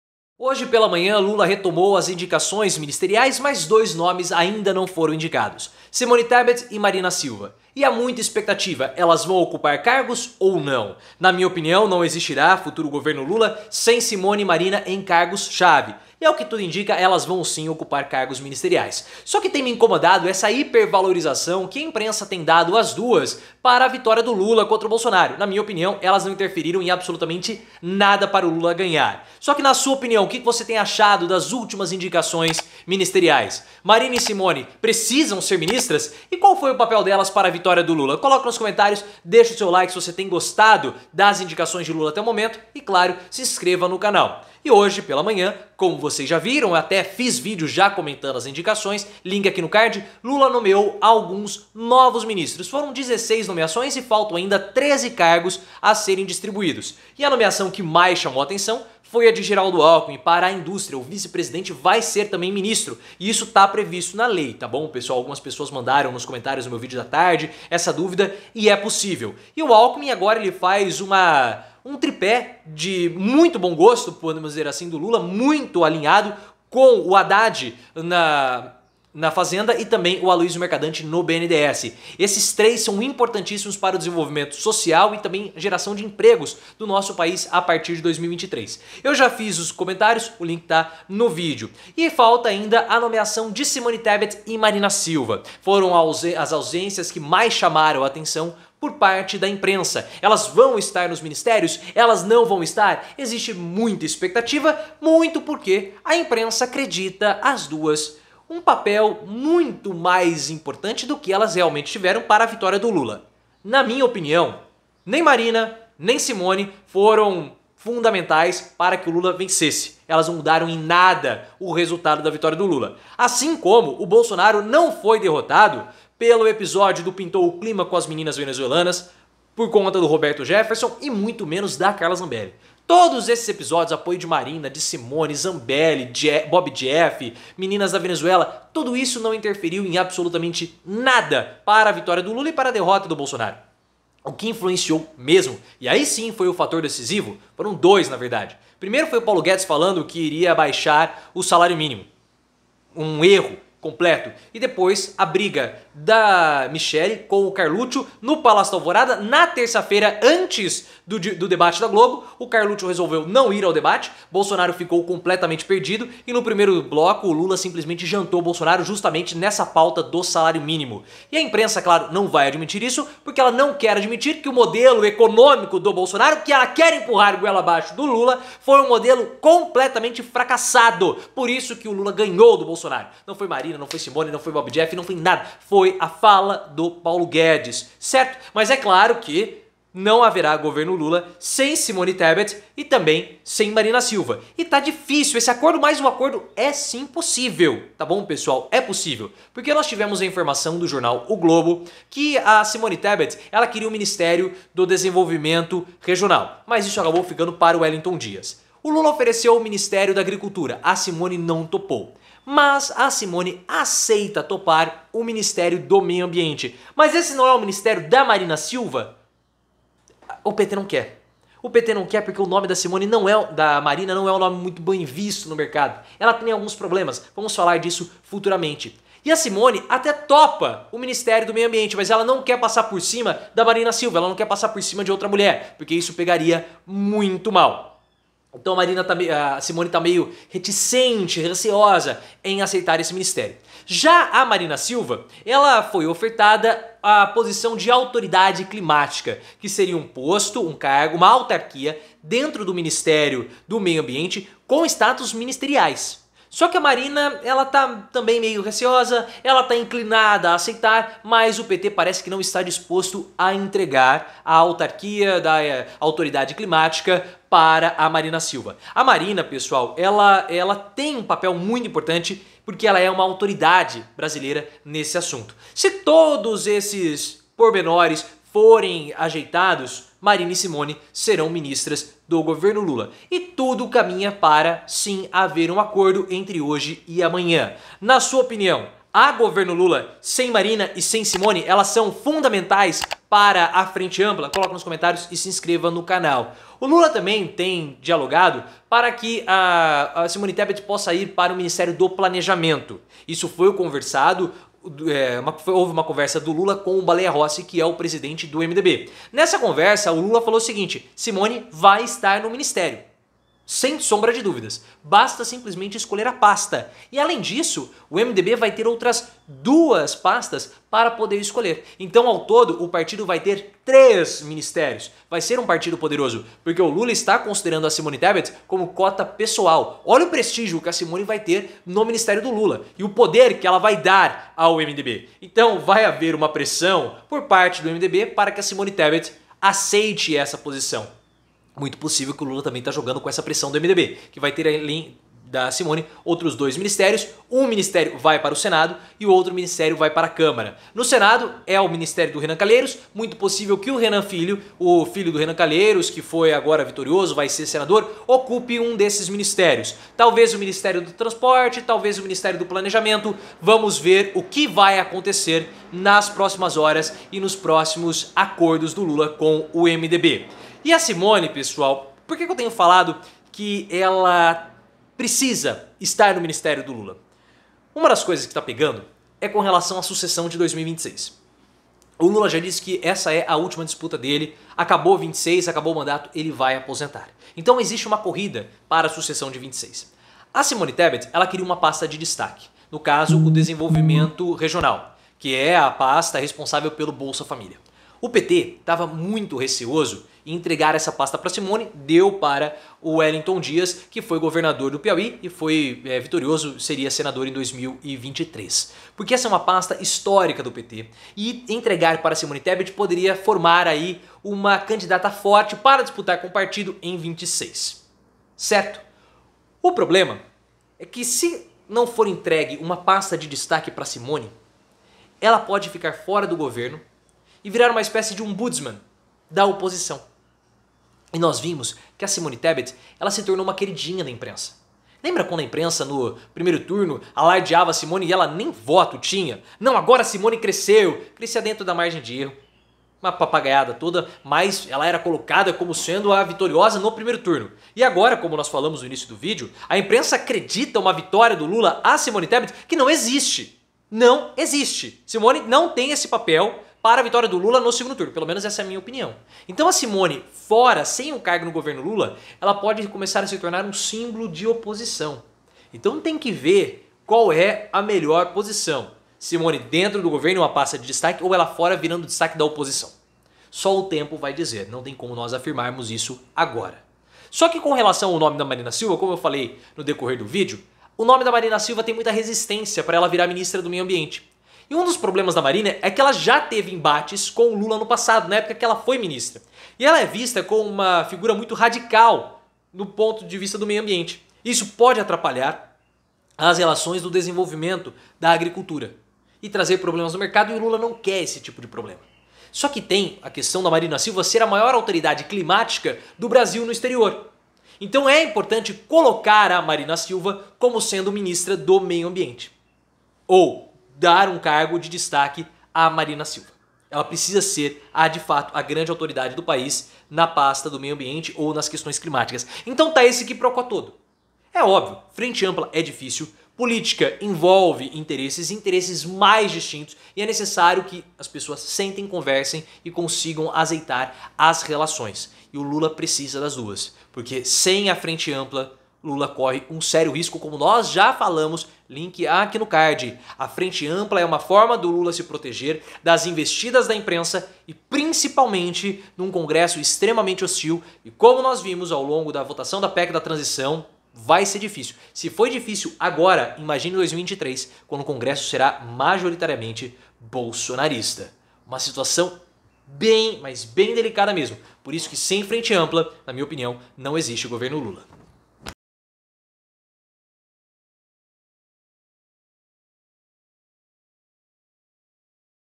💣B0MBA! LULA TEM ACORDO COM SIMONE! Ótima notícia!
Lula voltou a nomear novos ministros, mas ainda não anunciou oficialmente Marina Silva e Simone Tebet. Os nomes das duas sofrem resistências no PT, mas precisarão integrar algum ministério ou não haverá governo Lula. Um acordo está sendo encaminhado com uma dobradinha entre as duas no Meio Ambiente, mas nada está definido. Simone seria uma ministra da cota pessoal de Lula, fortalecendo o poder do MDB no futuro governo.

00:00 Simone e Marina fora?
Lula retomou a indicações dos ministérios, mas ainda não nomeou Marina Silva e Simone Tebet, provocando expectativa na imprensa. Sem Marina Silva e sem Simone Tebet, Lula não irá conseguir governar. Entretanto, apesar das dificuldades, tudo indica que ambas serão ministras.

00:44 Acerto com Alckmin
A escolha por Alckmin na Indústria é um certo de Lula, pois coloca alguém alinhado estrategicamente com as intenções do futuro presidente para o desenvolvimento social e laboral do país. Com Alckmin na Indústria, Lula conseguiu fechar uma espécie de alinhamento de setores importantes, incluindo a aprovação da PEC da Transição e Aloisio Mercadante no BNDES. São pessoas da alta confiança de Lula e poderão apresentar uma sinergia refinada para o futuro governo e para o desenvolvimento social do país.

01:32 Simone e Marina não foram decisivas
Apesar de a imprensa alardear que Simone Tebet e Marina Silva foram essenciais para a vitória de Lula, seus apoios são superestimados. Lula não venceu por causa de nenhuma das duas, assim como Bolsonaro não perdeu por causa de Roberto Jefferson, das meninas venezuelanas e de Carla Zambelli. Lula venceu e Bolsonaro perdeu pelas ameaças de Paulo Guedes referentes ao salário mínimo e pela disputa familiar entre Carluxo e Michelle que prejudicou o atual presidente no debate da Globo.

03:09 Acordo é possível
Apesar de todos os entraves, existe uma possibilidade de acordo. Simone Tebet foi convidada a ocupar o Ministério do Meio Ambiente, mas está relutante porque não quer passar por cima de Marina Silva. Caso Marina aceite ocupar o cargo da autoridade climática, uma secretaria com status de ministério. Marina defende que a autarquia seja subordinada ao ministério do Meio Ambiente. Não há uma definição sobre o caso. Se todas as partes toparem, Marina e Simone estarão no governo Lula.

04:56 Simone no Planejamento
De acordo com o O Globo, Lula informou a Baleia Rossi, presidente do MDB, que Simone Tebet será sua ministra. Simone poderá escolher o Meio Ambiente ou o Planejamento. Além de Simone, o MDB deverá ter outras duas pastas, totalizando três ministérios. Renan Filho é um dos cotados. Assim, Simone Tebet entraria como cota pessoal de Lula e não partidária, aumentando ainda mais o seu prestígio.

06:09 O papel de Simone Tebet
O receio do PT com Simone Tebet é deixa-la em uma pasta de destaque, como a do Desenvolvimento Social, o ministério do Bolsa Família, e fortalece-la politicamente para 2026. Como Lula não irá disputar 2026, segundo suas próprias palavras, a corrida pela sucessão está aberta. Entretanto, se Simone ficar de fora, ela poderá ser vista como a líder da oposição, pois, mesmo sem ter a mínima chance de ir ao segundo turno, tornou-se a queridinha da imprensa no primeiro turno. Simone poderá ficar mais fortalecida se não estiver no governo. Só o tempo irá dizer o resultado.

07:57 Resistências a Marina
O nome de Marina Silva sofre resistências para ser ministra do Meio Ambiente por ser considerada demasiadamente radical em termos ambientalistas. O nome dela sofre muita resistência do mercado, embates já ocorreram quando Marina foi ministra da pasta no passado. Lula não quer ter esse tipo de problema atualmente. Entretanto, Marina é a autoridade nacional mais reconhecida no exterior sobre o tema.

08:46 Frente ampla é necessária
Será importante que o PT entenda o real cenário envolvendo Lula e Arthur Lira. A melhor forma de Lula conseguir uma blindagem mais poderosa é com a frente ampla. Os ministérios precisarão ser melhor distribuídos entre os demais partidos da base de Lula, como MDB, PSB, PSD e até mesmo o União Brasil. O PT resiste em entregar ministérios mais estratégicos, podendo desagradar a base e ruir a frente ampla que servirá de proteção a Lula.